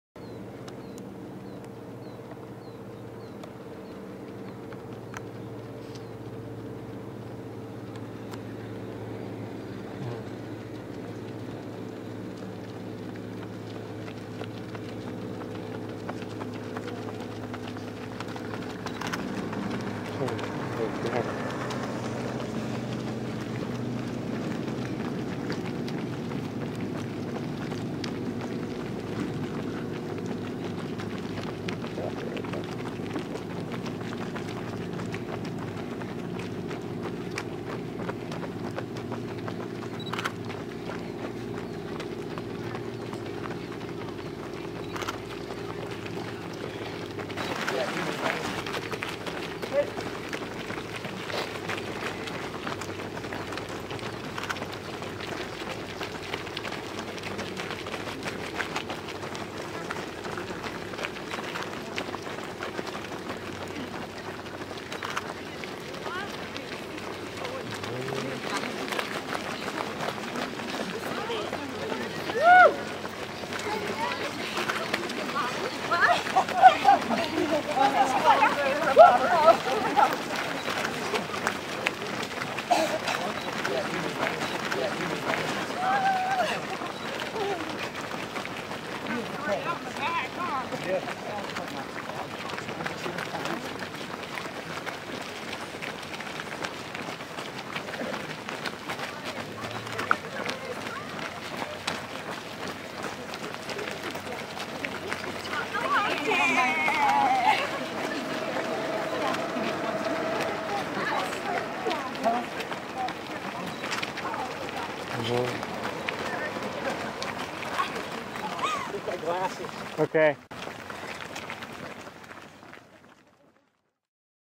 Rim2Rim 2013
Video of the runners coming out fo Bass Lake on the Rim2Rim Run 2013.  Thanks to Chad Dockstader for shooting the video while I ran.